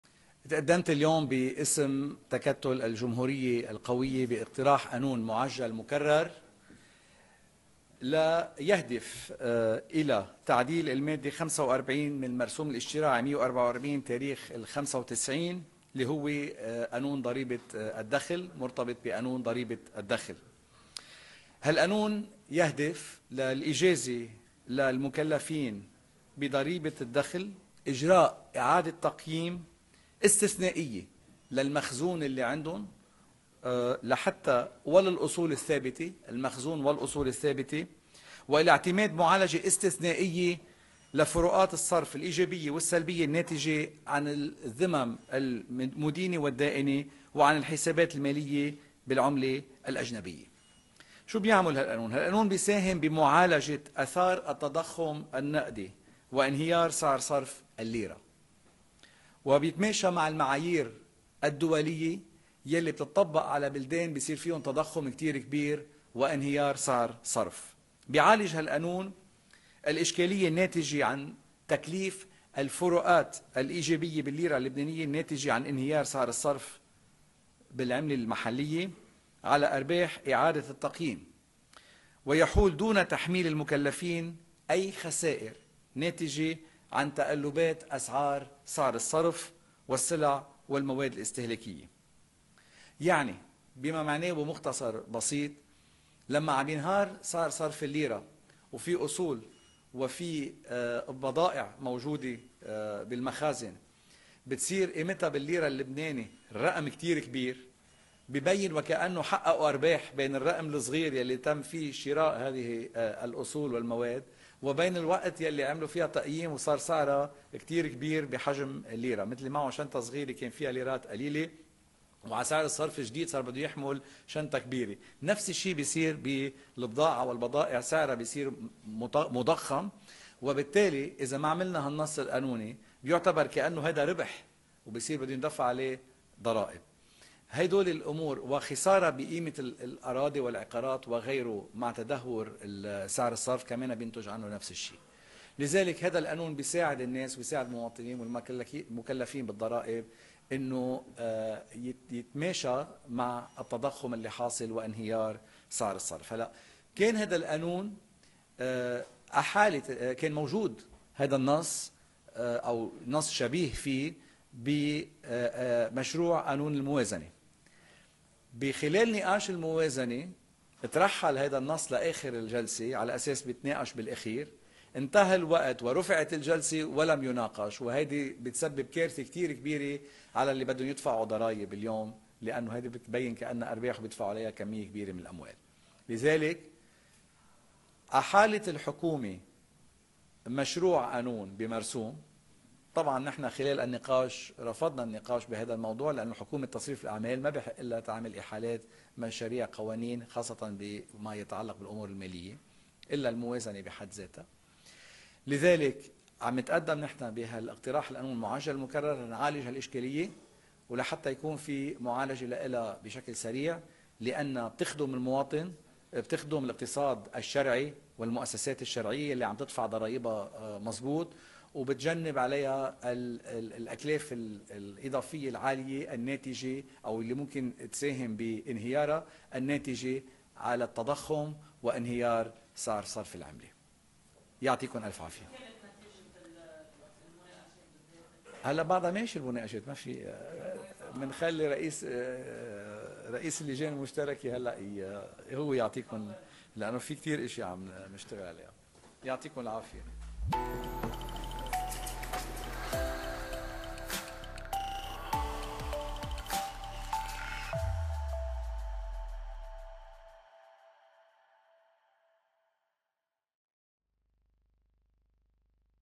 حاصباني : هذا القانون يساهم بمعالجة آثار التضخم النقدي وانهيار سعر صرف الليرة
تقدم النائب غسان حاصباني باسم تكتل الجمهورية القوية اقتراح قانون معجلا مكررا يرمي الى تعديل المادة 45 من المرسوم الاشتراعي 144 تاريخ 12/6/1995 (قانون ضريبة الدخل) والاجازة للمكلفين بضريبة الدخل إجراء اعادة تقييم استثنائية للمخزون وللأصول الثابة والى اعتماد معالجة استثنائية لفروقات الصرف الايجابية والسلبية الناتجة عن الذمم المدينة والدائنة وعن الحسابات المالية بالعملة الأجنبية.

وقال في مؤتمر صحافي ان "هذا القانون يساهم بمعالجة آثار التضخم النقدي وانهيار سعر صرف الليرة، ويتماشى مع المعايير الدولية الخاصة بالتضخم للاقتصادات التي تعاني من ارتفاعه".

اضاف:"يعالج  هذا القانون الاشكالية الناتجة عن تكليف الفروقات الايجابية بالليرة اللبنانية الناتجة عن انهيار سعر صرف الليرة بالضريبة على أرباح إعادة التقييم، ويحول دون تحميل المكلفين اية خسائر ناتجة عن تقلبات أسعار السلع والمواد الاستهلاكية".

 واشار الى ان" هذا النص قد ورد في مشروع الموازنة لكنه سقط بسبب ترحيله الى آخر لجلسة وثم سقط بسبب رفع الجلسة قبل مناقشته".

كما  لفت الى "ان الحكومة كانت قد أحالت مشروع قانون يرتبط بالموضوع، لكننا رفضنا مناقشته لعدم جواز احالة قوانين من حكومة تصريف أعمال. ولأهمية الموضوع في حماية المكلفين بعد انهيار سعر الصرف، تم تقديم الاقتراح المعجل المكرر من قبل تكتل الجمهورية القوية لاقراره ضمن الآلية الدستورية".